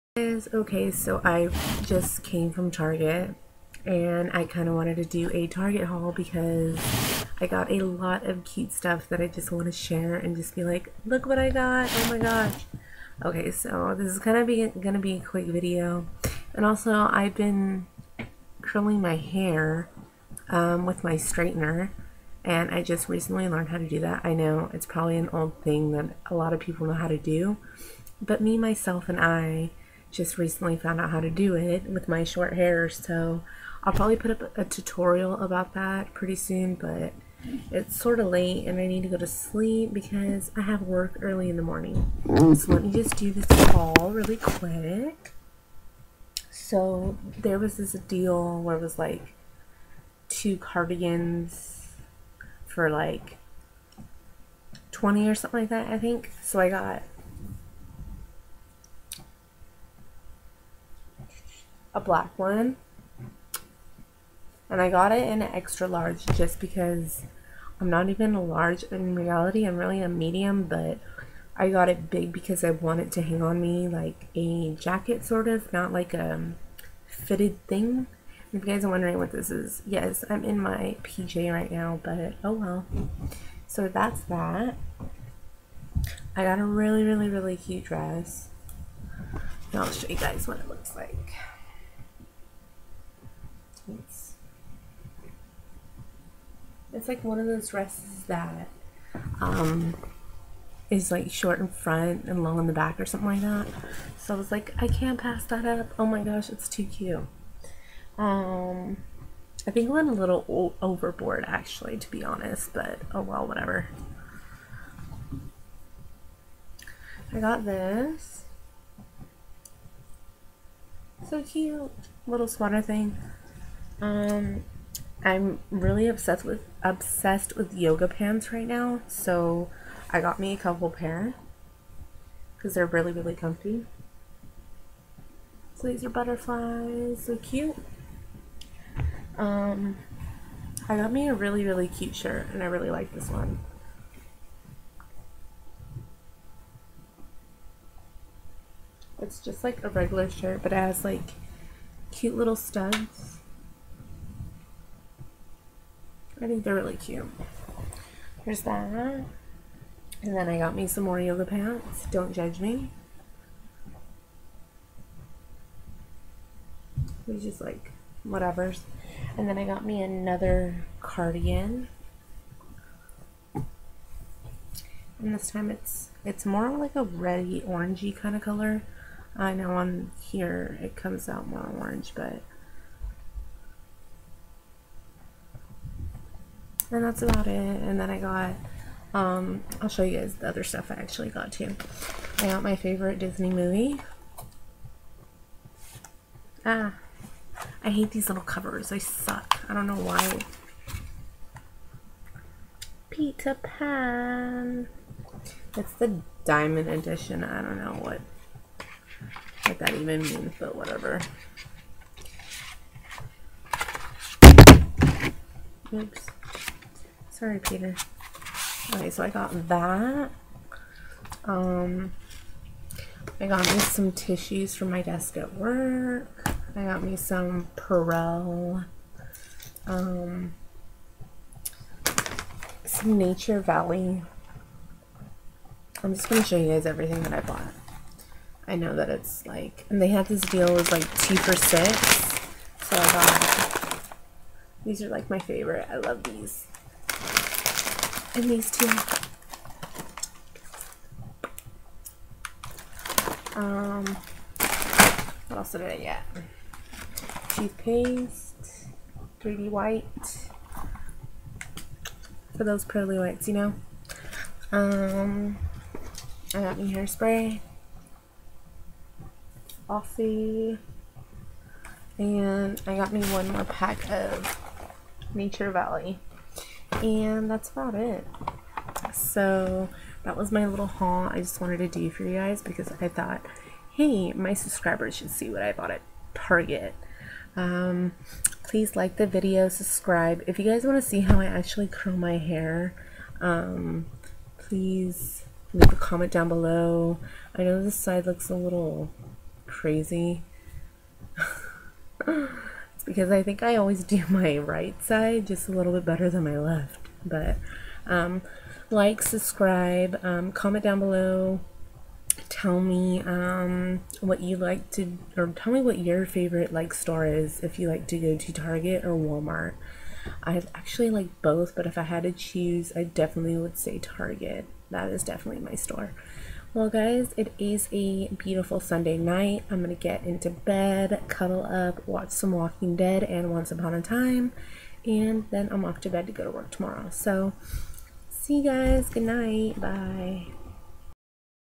target HAUL
i went to target and got a few things i really didnt need lol